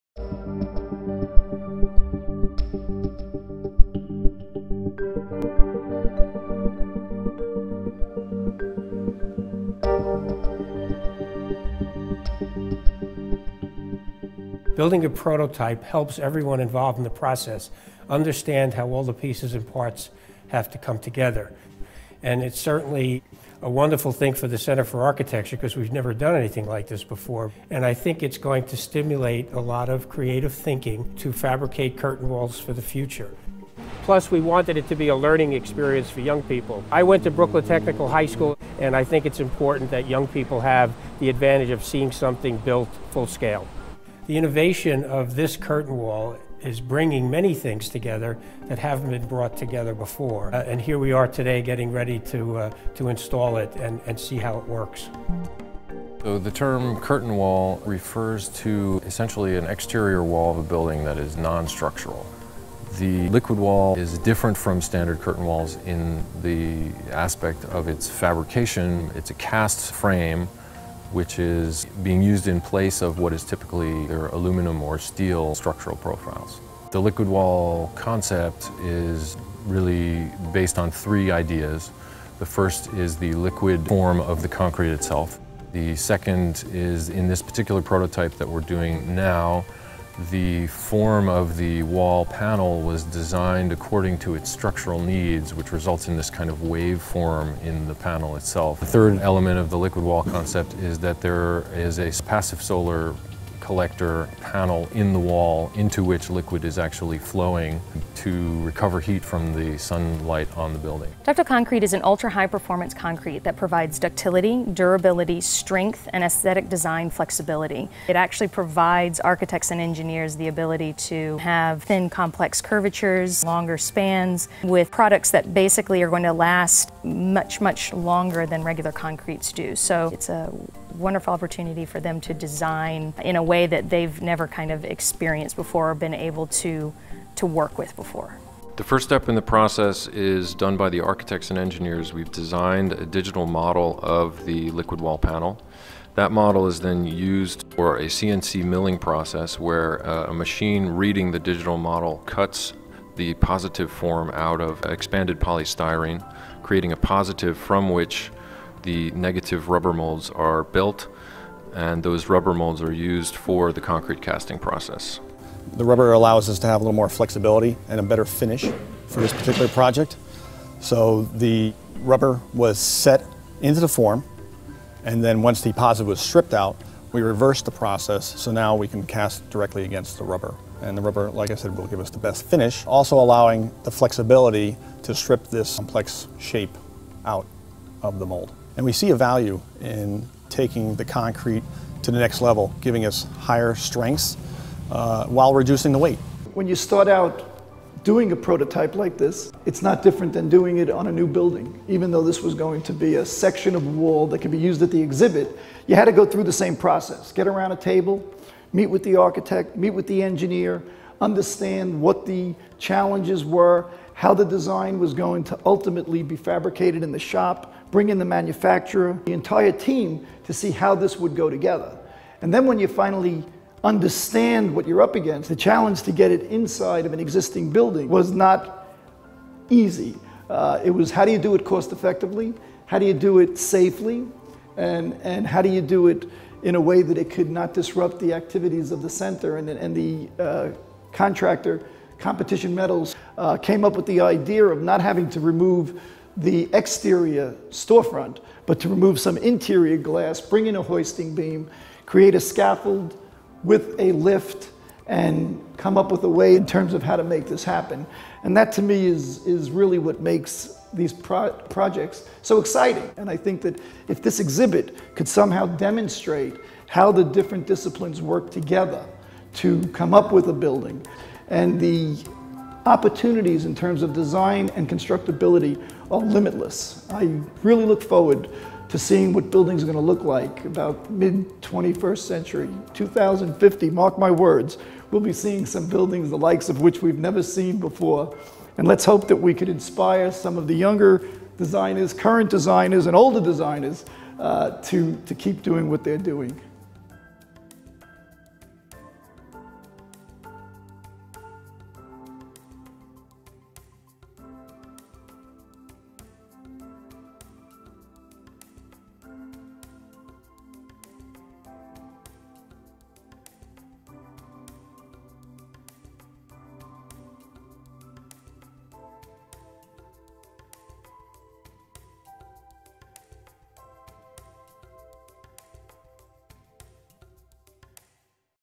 The Liquid Wall Prototype 2010
The Liquid Wall is an architecturally versatile, energy-positive, unitized curtain wall system. Cast in fiber-reinforced ultra-high-performance concrete, the Liquid Wall combines economical, custom-designed mass production with long-lasting, versatile and upgradable façade components.